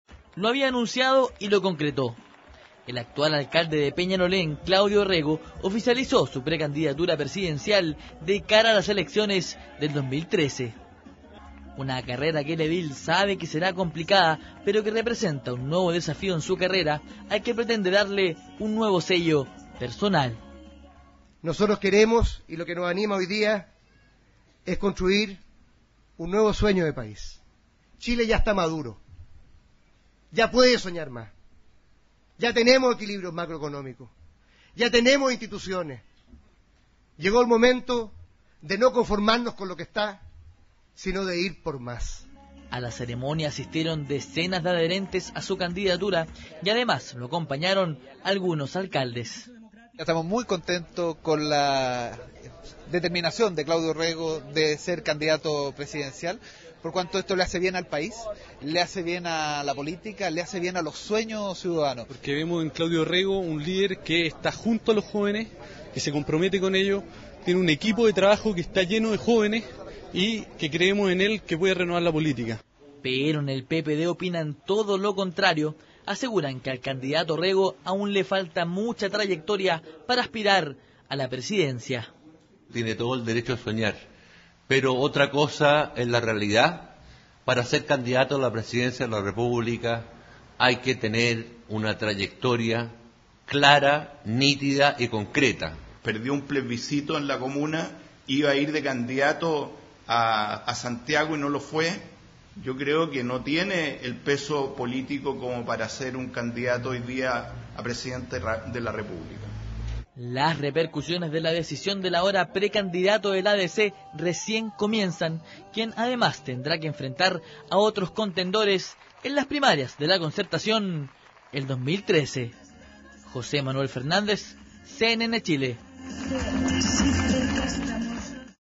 Claudio Orrego lanzó candidatura presidencial
Con éste, ya son tres los candidatos DC que apuestan a competir para las primarias en miras de las elecciones este 2014.